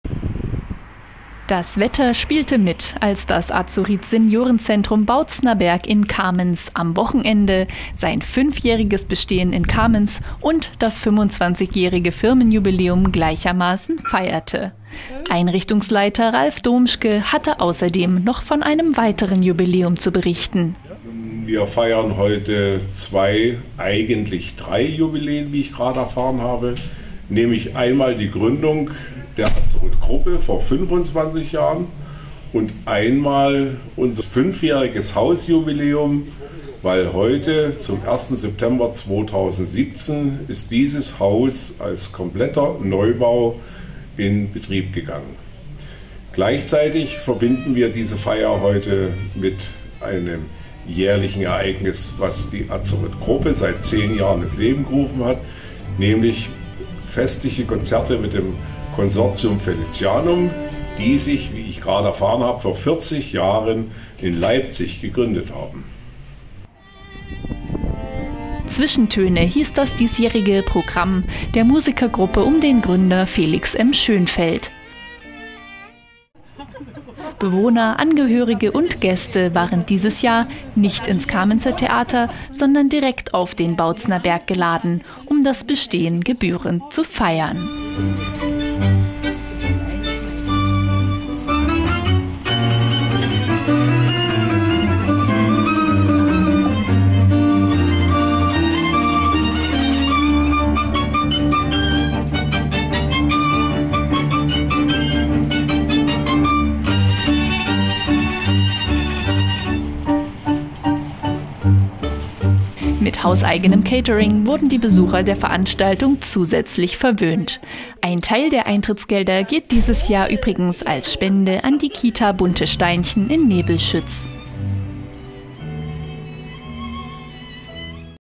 Kamenz: Azurit feiert mehrfaches Jubiläum - LAUSITZWELLE
Das Wetter spielte mit, als das AZURIT Seniorenzentrum Bautzner Berg in Kamenz am Wochenende sein fünfjähriges Bestehen in Kamenz und das 25-jährige Firmenjubiläum gleichermaßen feierte. Einrichtungsleiter Ralph Domschke hatte außerdem noch von einem weiteren Jubiläum zu berichten.